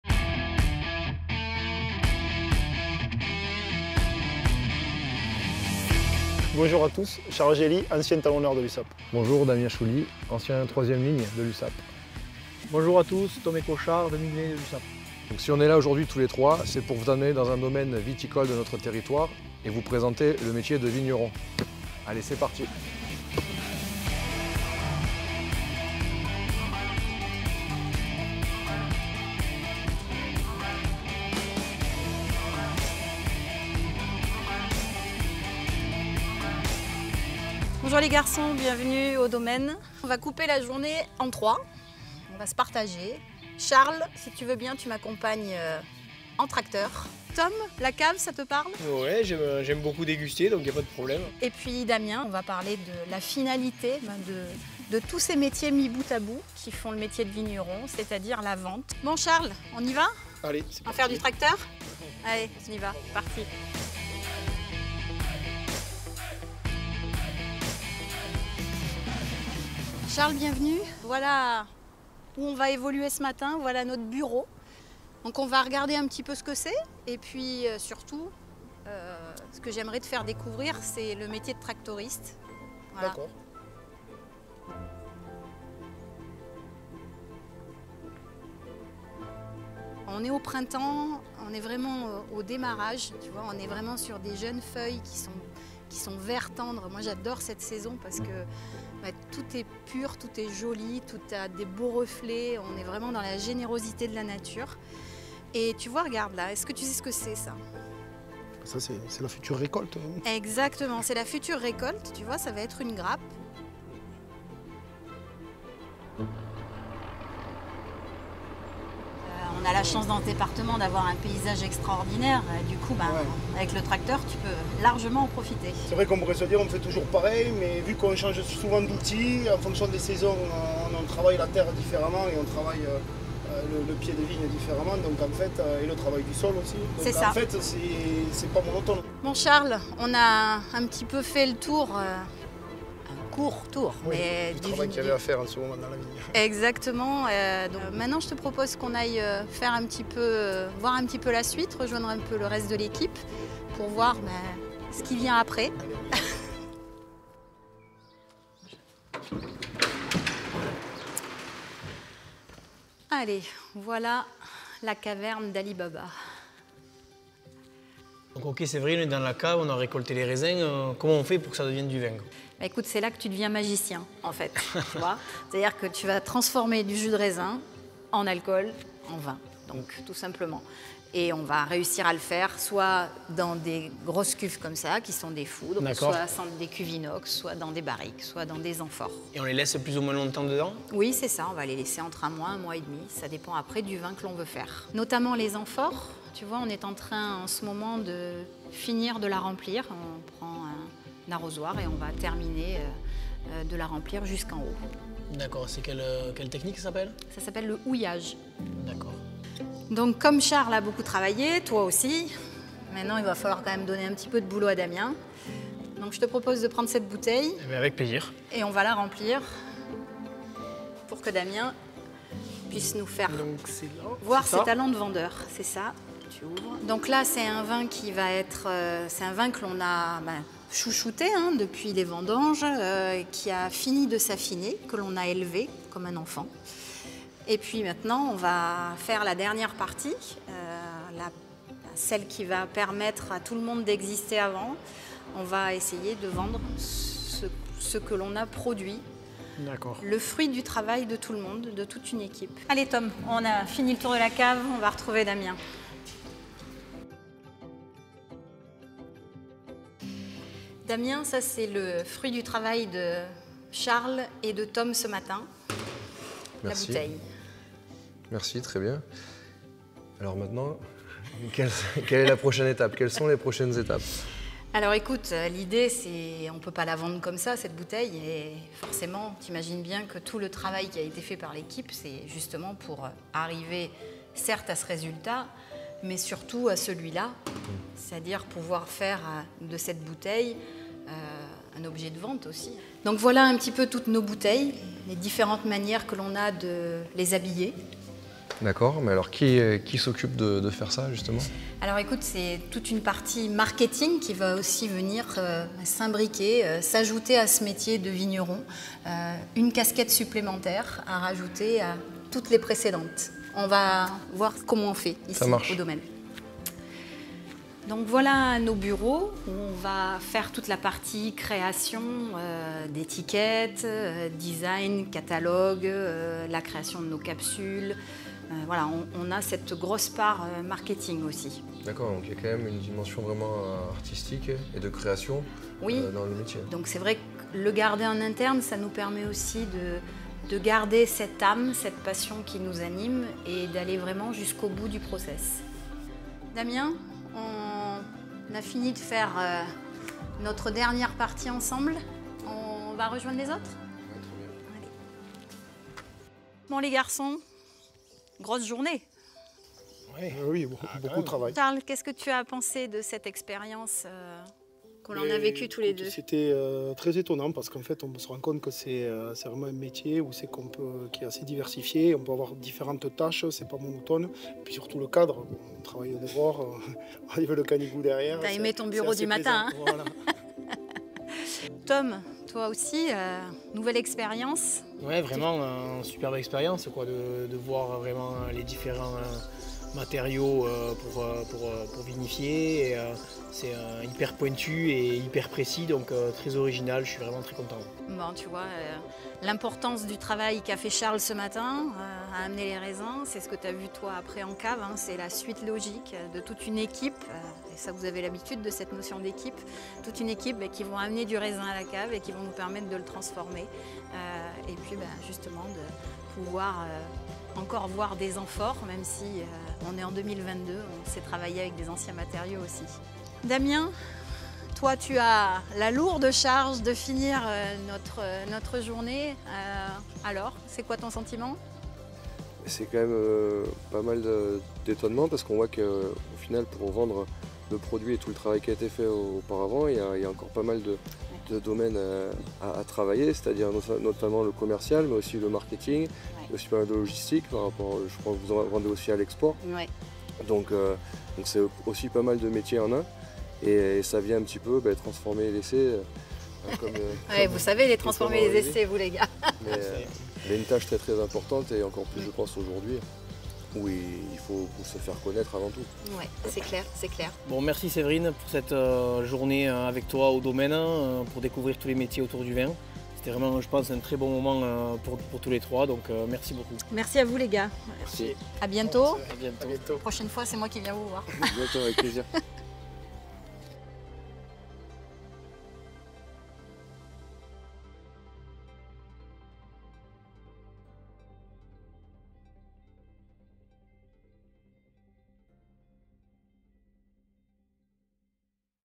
Les joueurs de l’USAP à la découverte des métiers du vin !
Dans le cadre du projet Mar I Vi réalisé par USAP Solidaire, le Fonds de Dotation du club, plusieurs joueurs et anciens joueurs de l’USAP ont eu l’opportunité de découvrir les métiers de notre territoire. Dans ce premier épisode, Damien Chouly, Charles Géli et Tom Ecochard sont allés à la rencontre des métiers du vin !